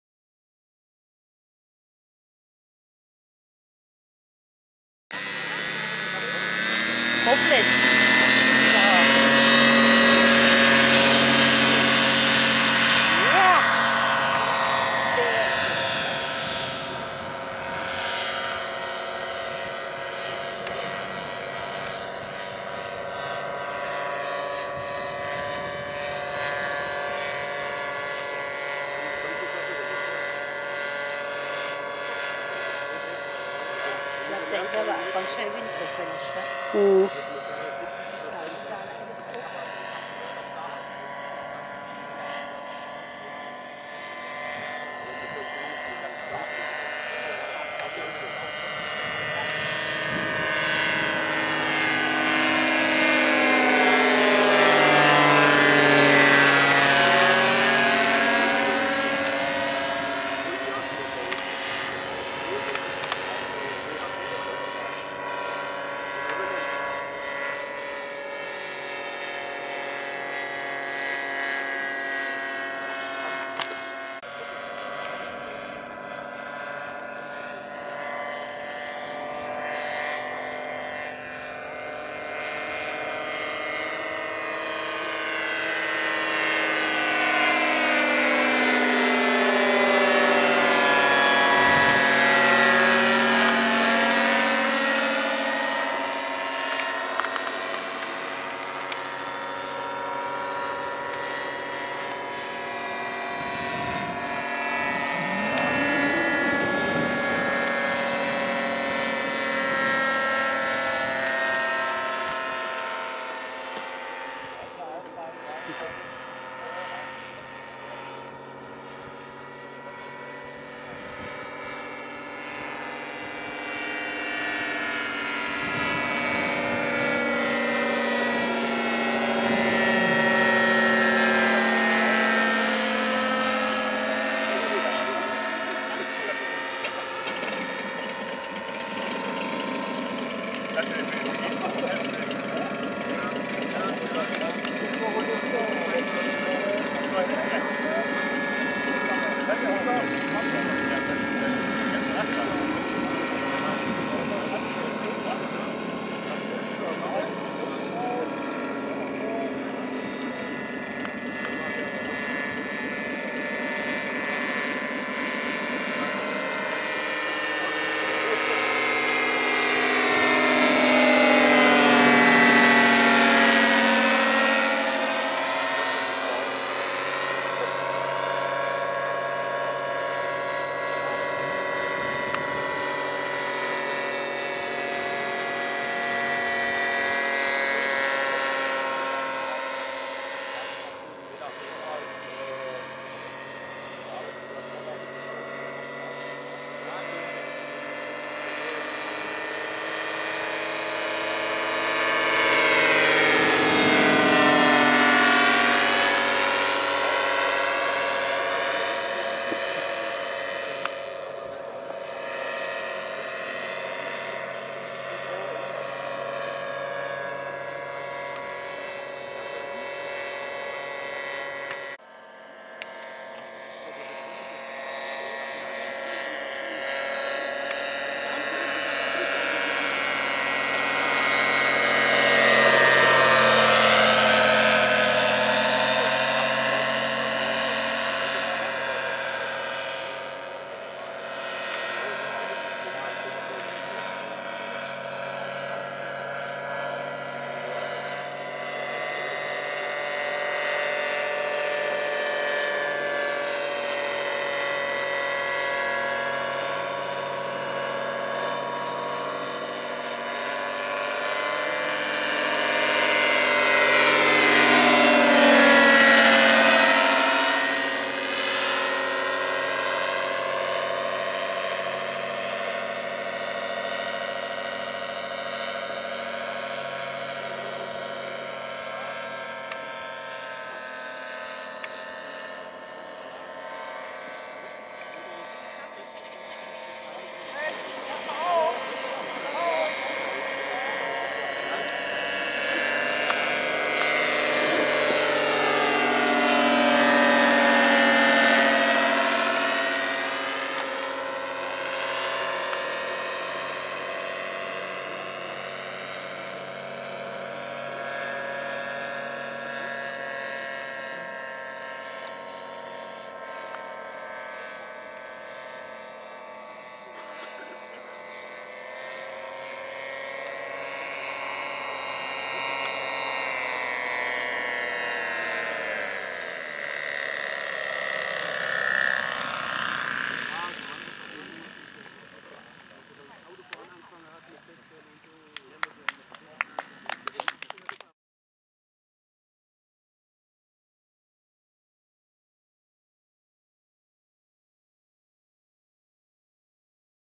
BEAUTIFUL RC BIPLANE AIRCO DH.2 (Doppeldecker Fly Out 2021 in Trebur)
Eigenbau einer Airco DH.2
Spannweite:
Gewicht:
Motor:

geflogen beim Doppeldecker Fly Out 2021 in Trebur.